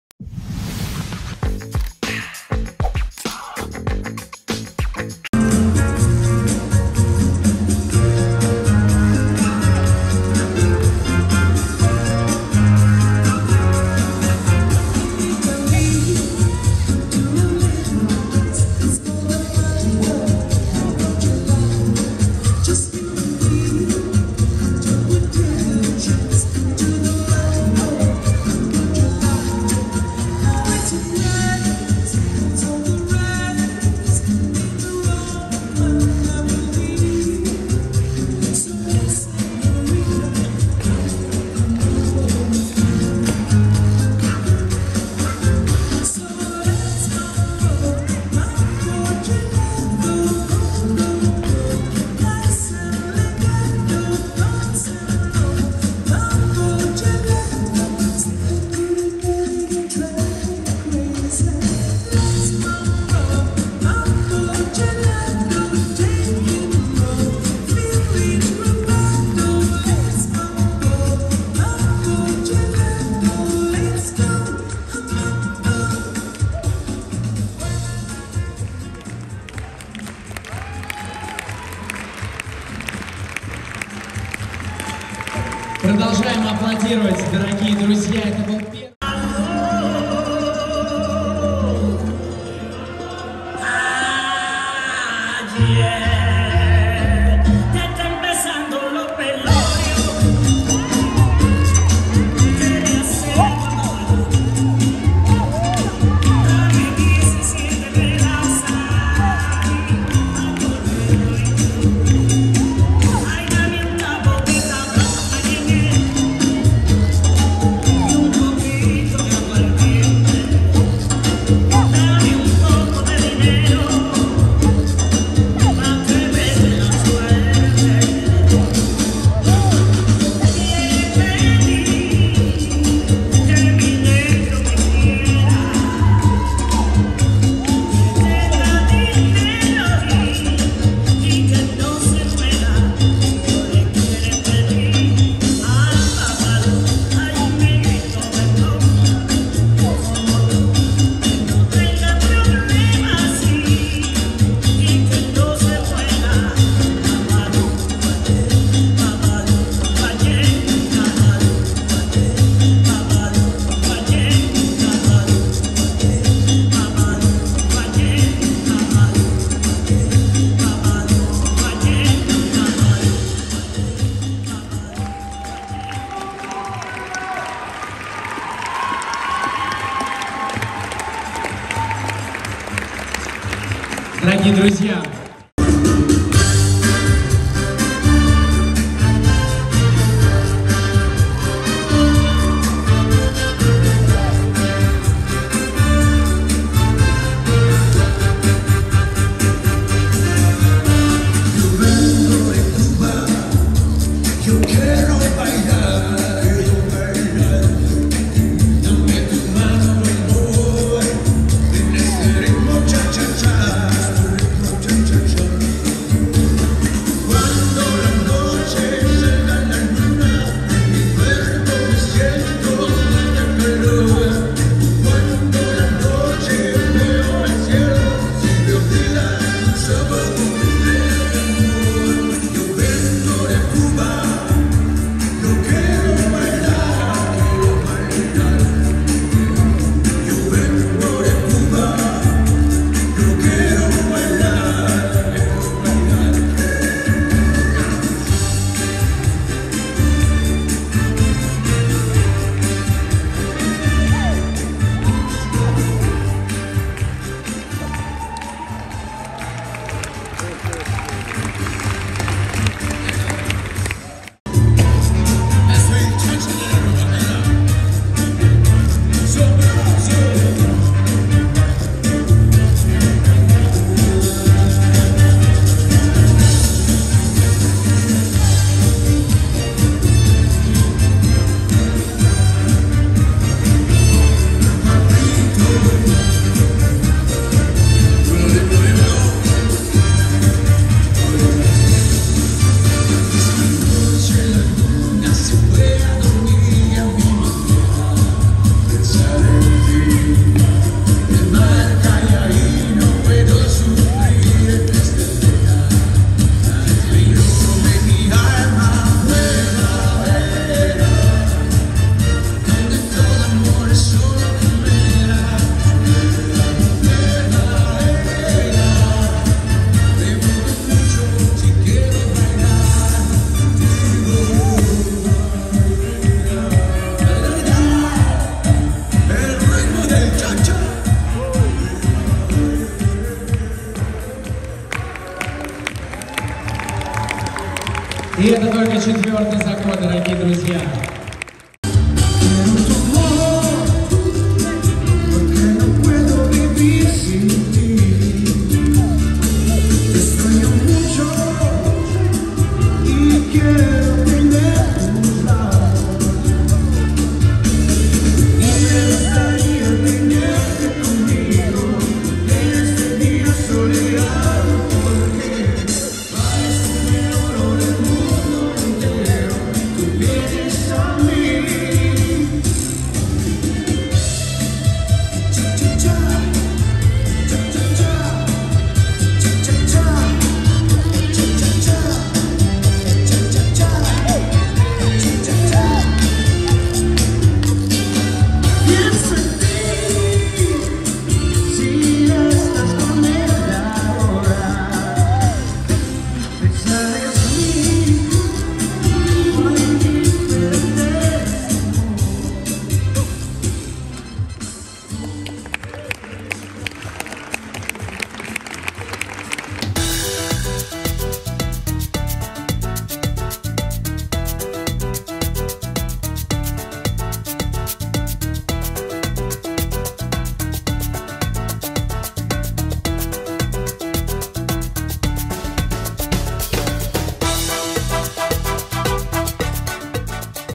Cha Cha Cha - Amateur Latin - 1/16 Final I Crystal Ball 2023
Amateur International Latin Championship - 1/16 Final - Cha Cha Cha
Thank you to the organizers of Crystal Ball 2023  for this wonderful event!